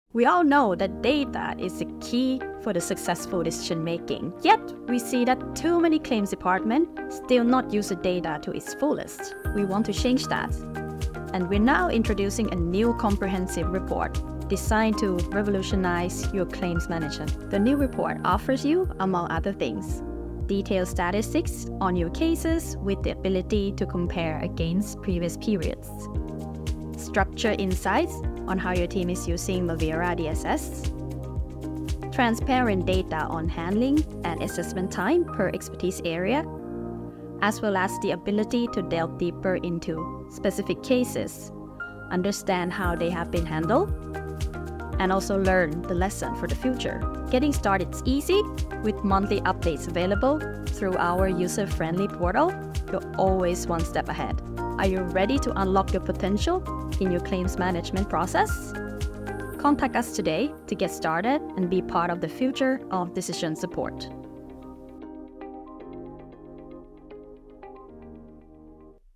Mavera claims report
For every claims leader, it's crucial to constantly explore ways to enhance claims management, ultimately aiming to reduce both claims handling costs and claims costs. The key to improvement? Rich insights and structured statistics.

Together with our customers, we have developed a new claims report with statistics to empower your decision-making.

In this brief video, Palita demonstrates how the report can improve your claims management, offering a sneak peek at some of the key statistics featured.